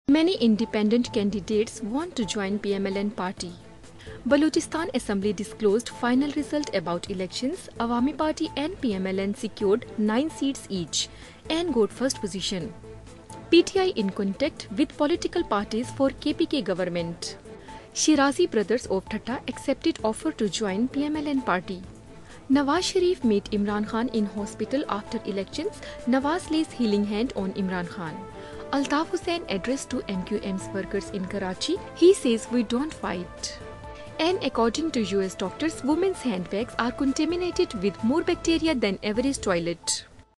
Tribune headlines: 1pm, May 15, 2013
Independent candidates express desire to wish PML-N.

PTI in contact with political parties to form government in K-P.

Nawaz Sharif meets Imran Khan in Shaukat Khanum Hospital.